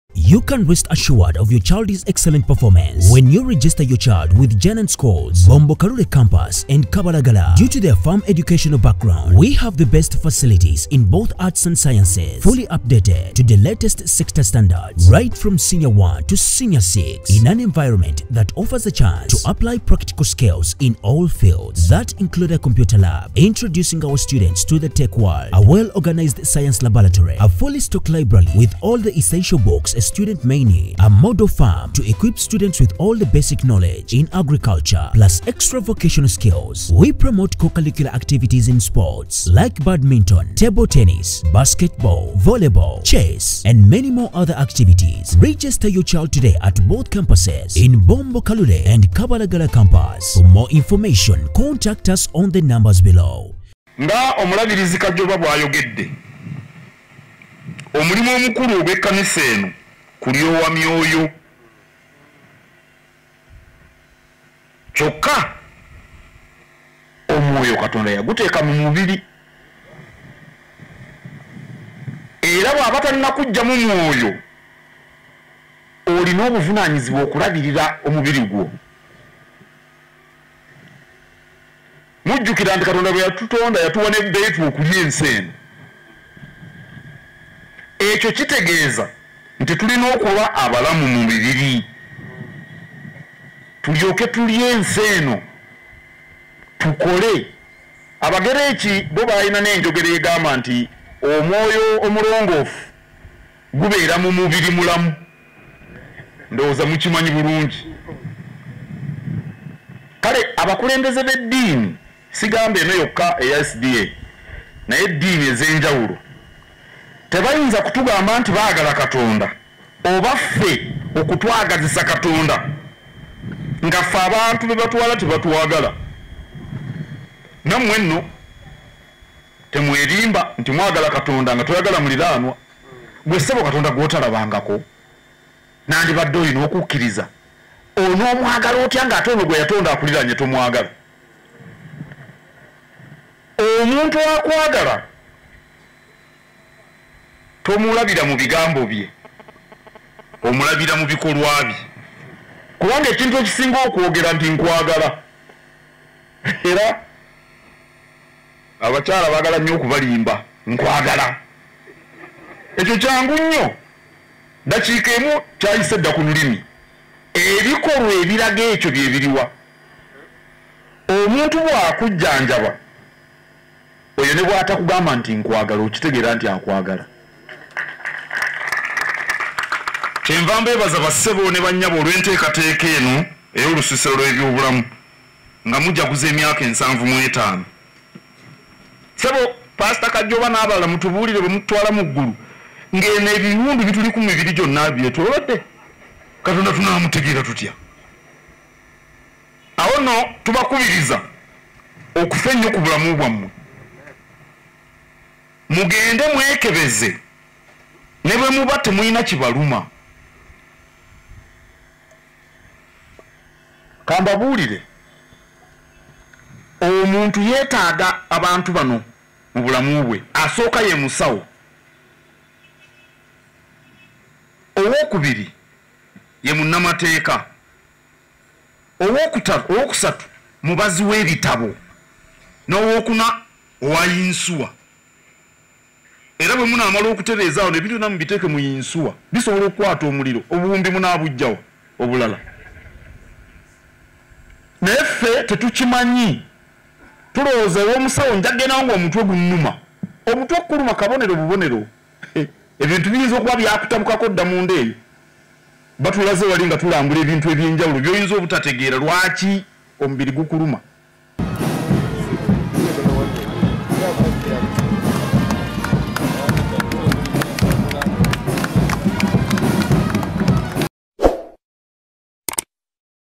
KATIKKIRO URGES UGANDANS TO GO FOR REGULAR MEDICAL CHECK UPS
Katikkiro Charles Peter Mayiga akubiriza bannayuganda bulijjo okujjumbira okwekebeza buli kadde kibayambe okwetaasa ku ddwadde ezimu.